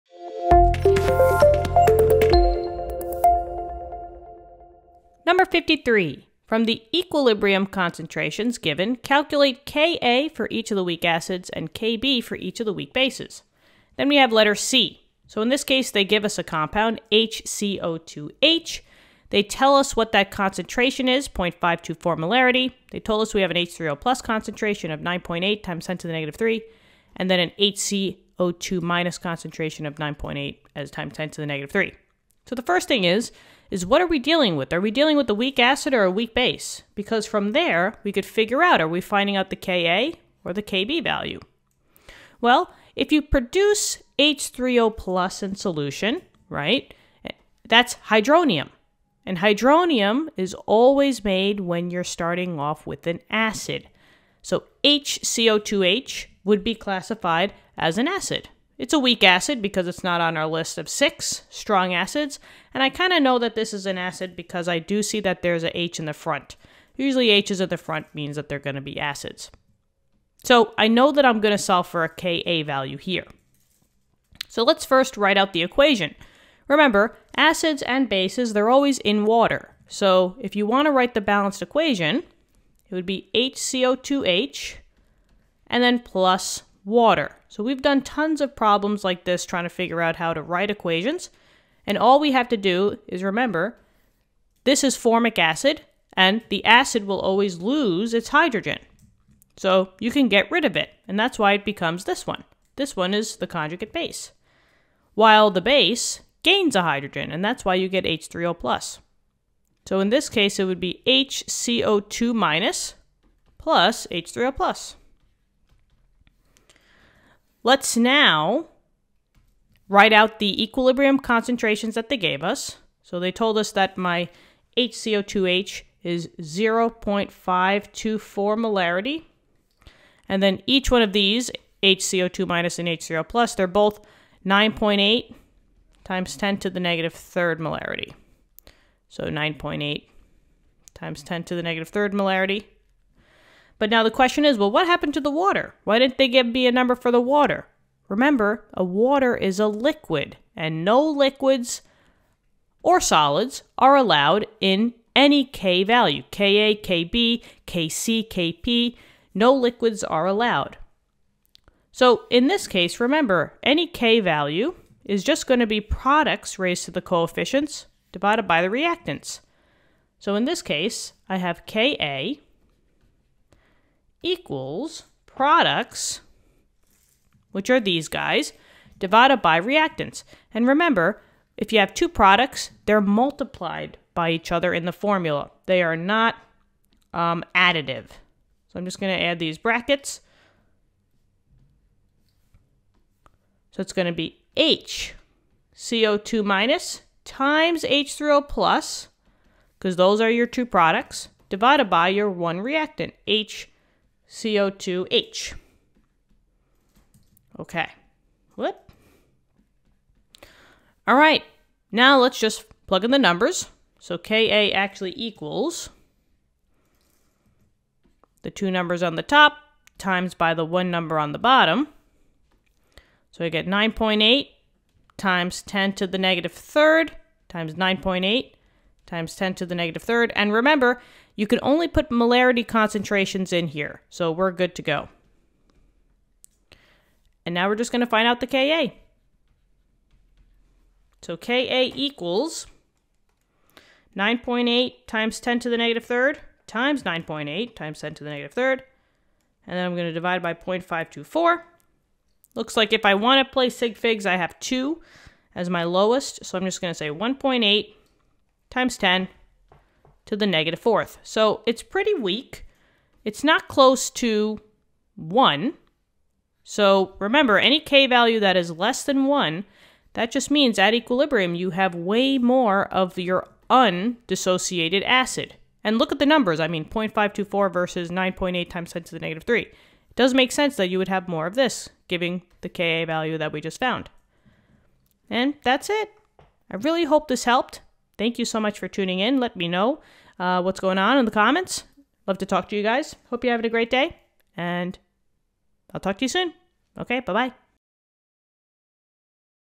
14.53c | How to calculate the Ka for HCO2H from equilibrium concentrations
From the equilibrium concentrations given, calculate Ka for each of the weak acids and Kb for each of the weak bases.
HCO2H: [HCO2H] = 0.524 M
[H3O+] = 9.8 × 10^−3 M
[HCO2−] = 9.8 × 10^−3 M